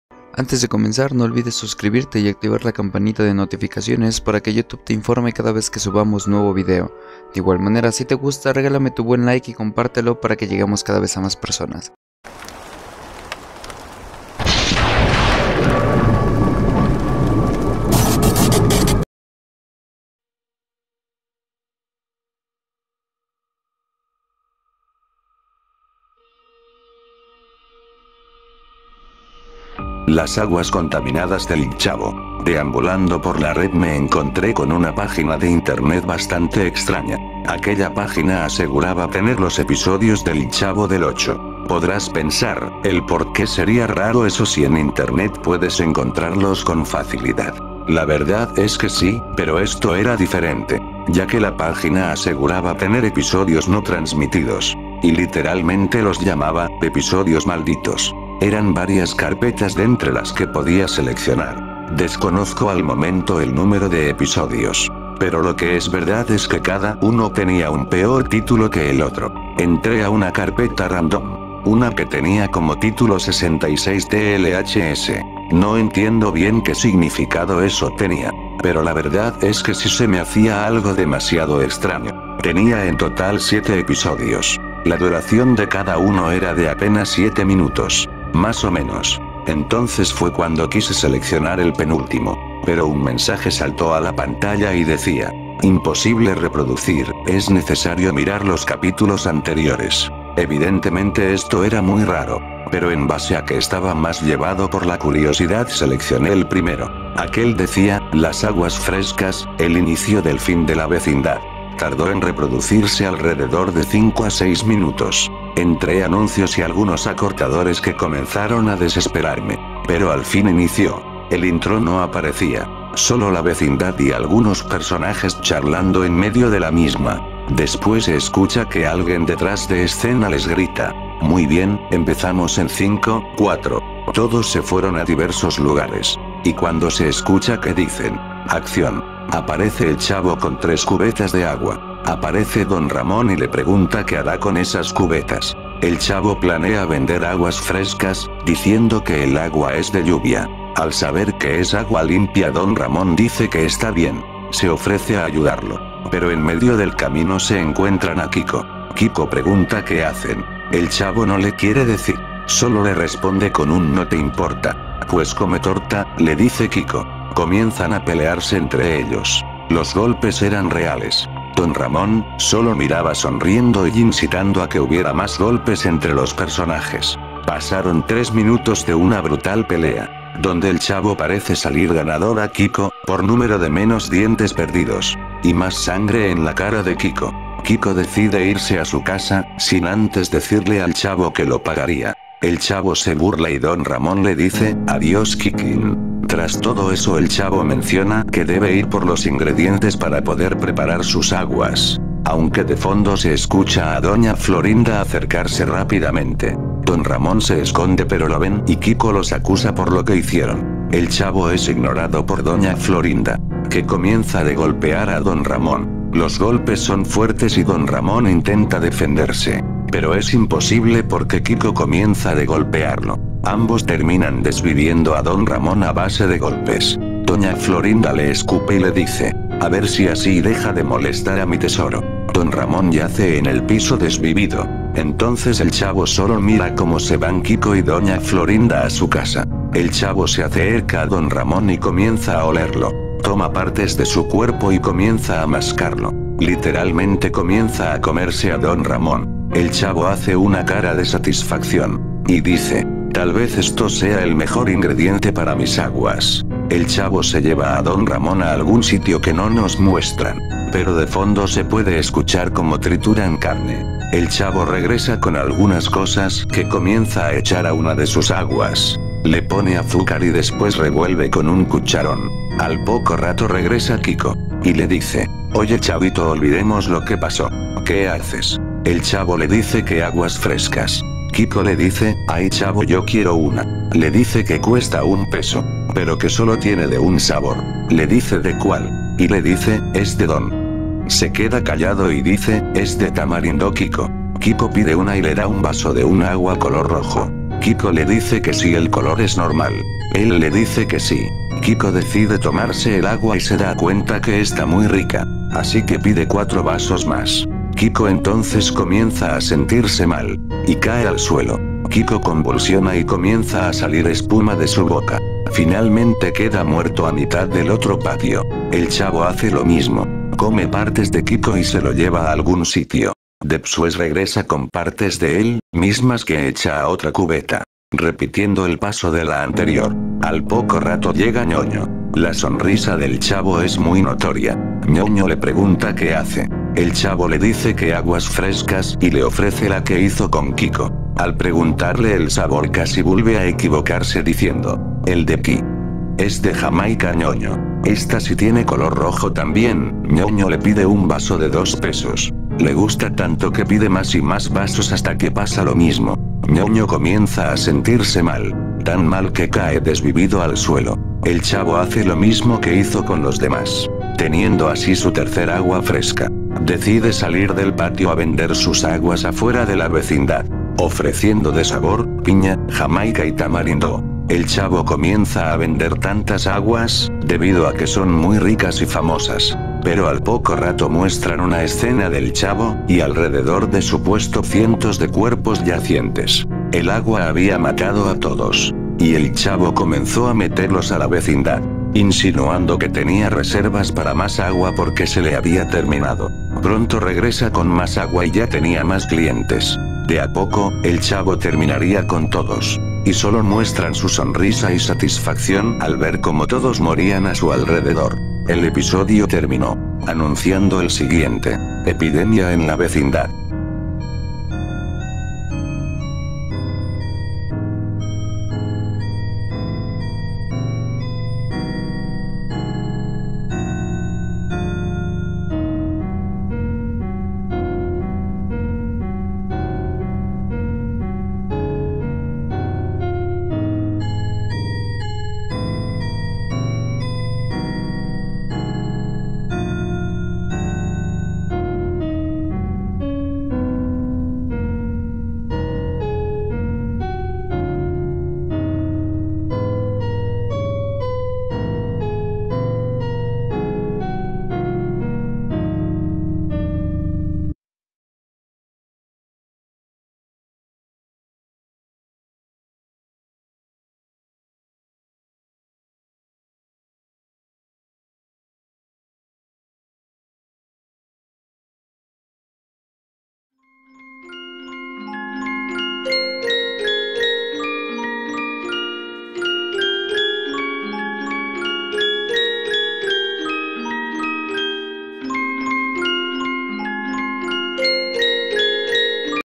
Las Aguas Frescas del Chavo, Creepypasta del Chavo del 8
Sinopsis: Una extraña página contiene en si una extraña cantidad de episodios que jamás se habías visto del chavo del 8, icónicos de título pero tan extraños desde que el espectador, puede ver un episodio horrible donde las aguas frescas son el principal problema.